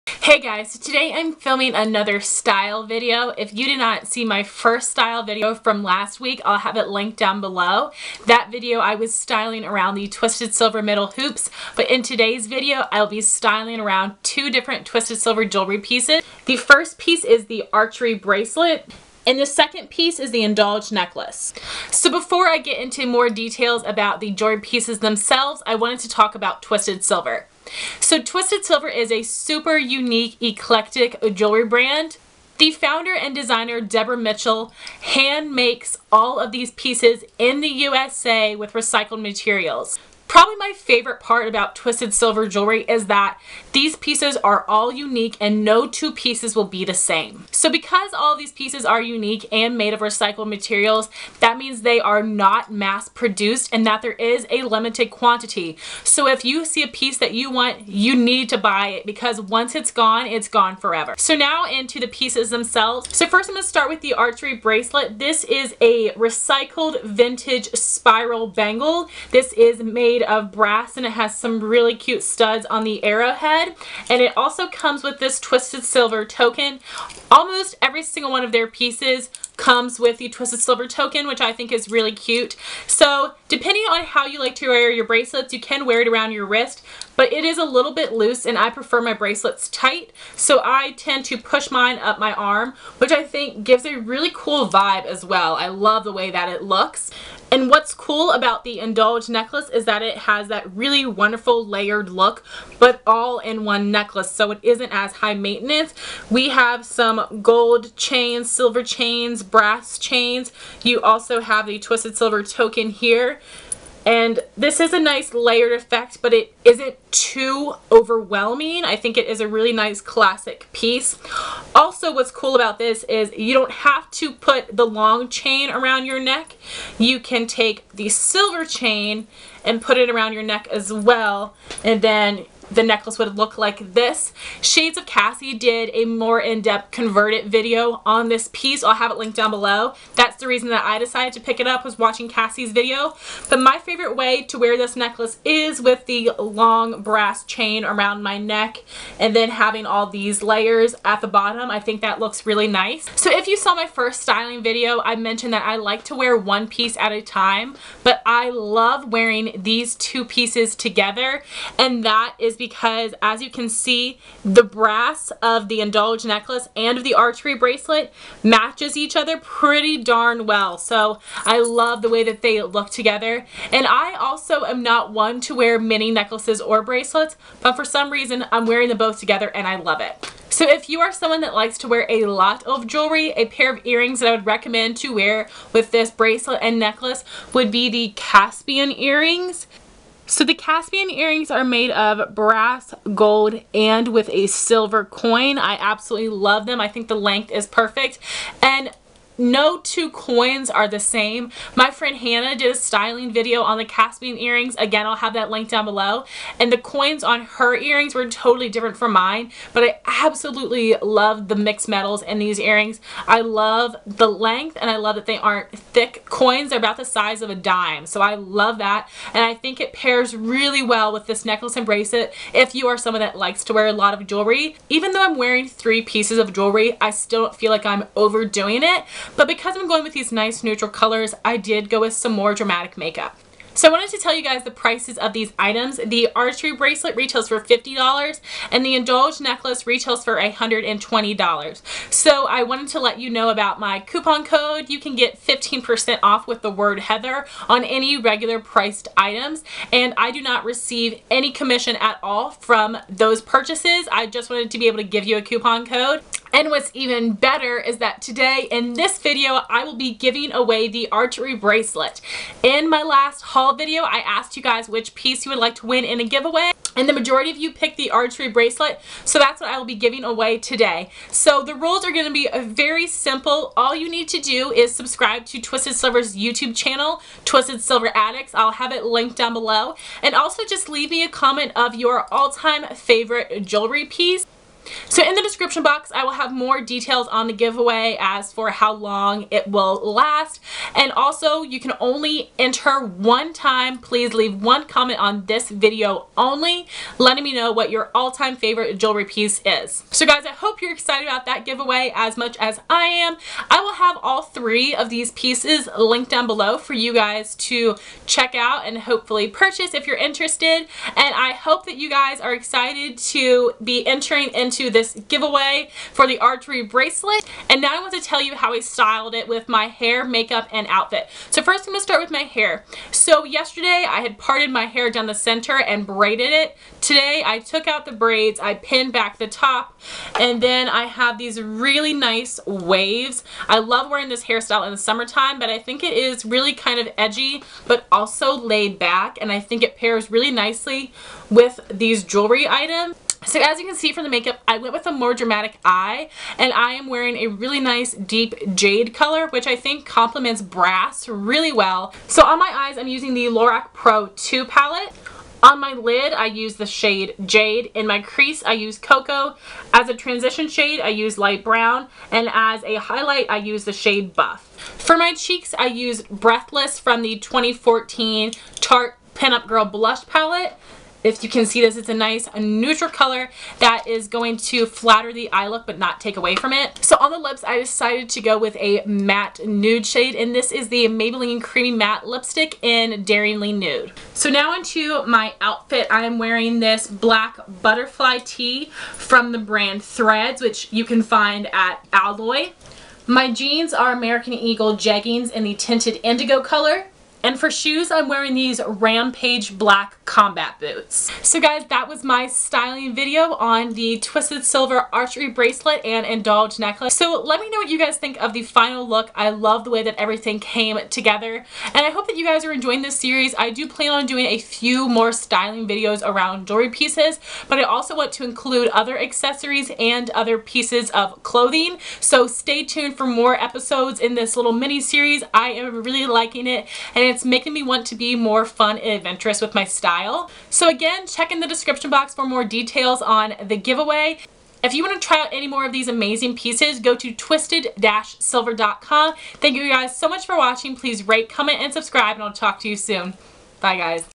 HOW I STYLE| Twisted Silver Archery Bracelet & Indulge Necklace
I chose the winner using random.org
The winner is ambeautylife

-Leave only ONE comment telling me your favorite jewelry piece
-Winner will be drawn through random.org

MAKEUP DETAILS
EYES- Lorac Pro 2 Palette (Jade, Cocoa, Light Brown, Buff)
CHEEKS- Tarte Breathless
LIPS- Maybelline Creamy Matte Lipstick in Daringly Nude

OUTFIT DETAILS
Shirt- Threads for Alloy
Jeans- American Eagle Jeggings in Tinted Indigo
Boots- Rampage for Belk

Camera: Nikon Cool Pix S4100
Video Editor: Windows Live Movie Maker